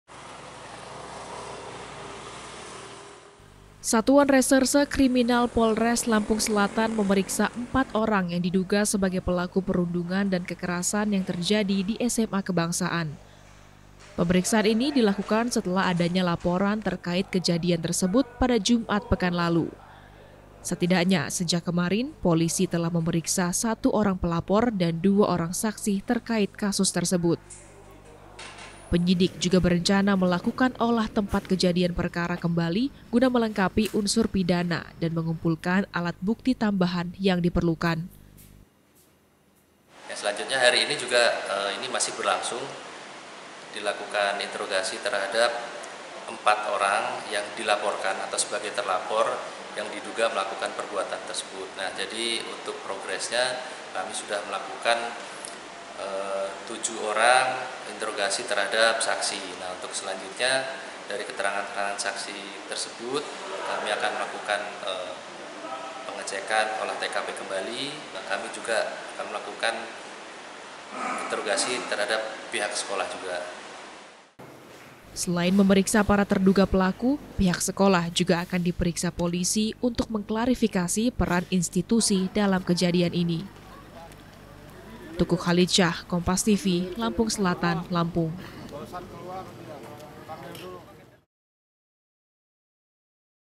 Perundungan di SMA Kebangsaan, 4 Terduga Pelaku Diperiksa Polisi
LAMPUNG, KOMPAS.TV - Satuan Reserse Kriminal Polres Lampung Selatan memeriksa 4 orang yang diduga sebagai pelaku perundungan dan kekerasan yang terjadi di SMA Kebangsaan.

Pemeriksaan ini dilakukan setelah adanya laporan terkait kejadian tersebut pada Jumat pekan lalu.

Setidaknya sejak kemarin polisi telah memeriksa 1 orang pelapor dan 2 orang saksi terkait kasus tersebut.

Penyidik juga berencana melakukan olah tempat kejadian perkara kembali guna melengkapi unsur pidana dan mengumpulkan alat bukti tambahan yang diperlukan.

Selain memeriksa para terduga pelaku, pihak sekolah juga akan diperiksa polisi untuk mengklarifikasi peran institusi dalam kejadian ini.